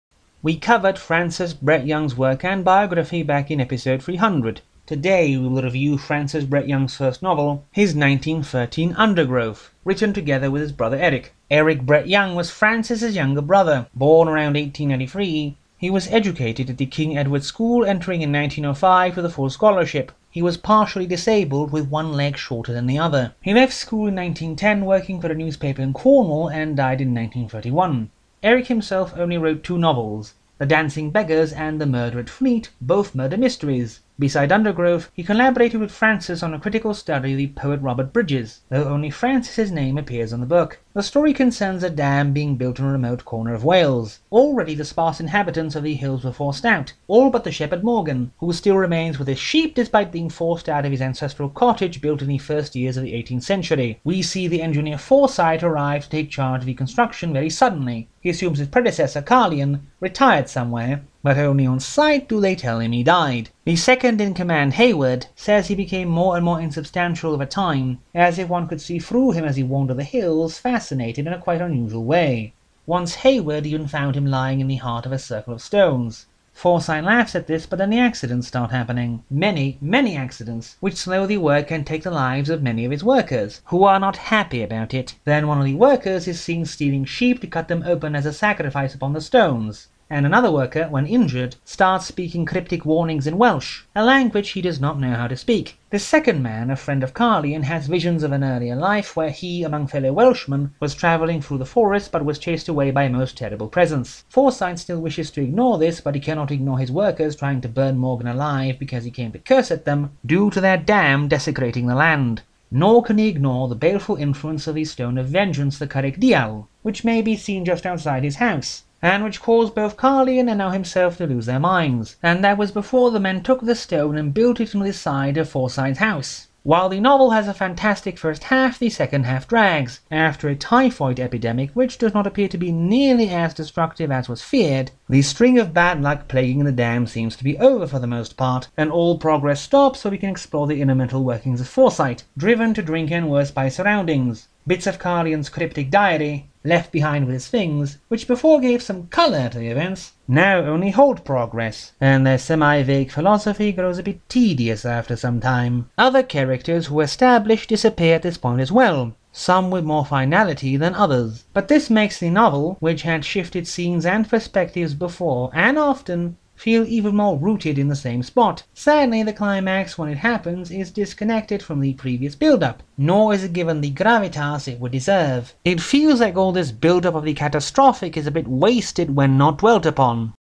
The Weird and the Wonderful CCCIX: Undergrowth by Francis and Eric Brett Young
Francis Brett Young remains at least somewhat remembered, usually for more general fiction, though he worked the occasional supernatural motiff into his work here and there, aside, of course, from "Cold Harbour". His brother Eric on the other hand seems to be an enigma, especially since he died at age 39 and only ever published two novels under his name, the ending to one I have seen described as "utterly predictable and unsatisfactory".

In 1913 however both Francis and Eric collaborated on "Undergrowth", their first work of fiction.

It is the story of a dam built in the wild hills of Wales, at a place described as truly and utterly pagan. And also a story of sacred stones, bloody sacrifices, terrible nightmares and a series of brutal accidents which thin the ranks of the workers considerably, all, so the illiterate shepherd Morgan seems to think, stemming from the supernatural vengeance of the Mountain itself, after it had been scarred and desecrated by the new dam.

The second image is in fact one of the only existing photos of Eric Brett Young, courtesy of the Francis Brett Young Society.